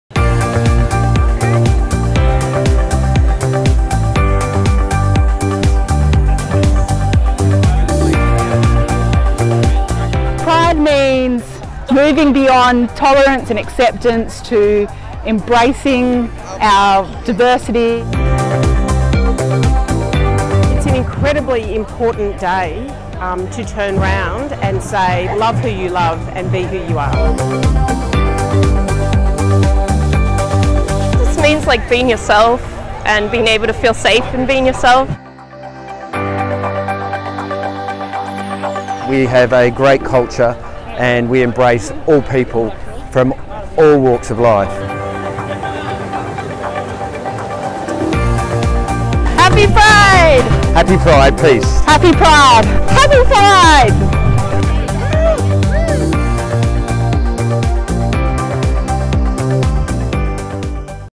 La Trobe joins Midsumma Pride March in solidarity with LGBTIQ students and staff
A 40-person contingent from La Trobe has donned rainbow flags and paraded through Melbourne alongside other Victorian universities in a show of love and support for same-sex attracted and gender diverse people.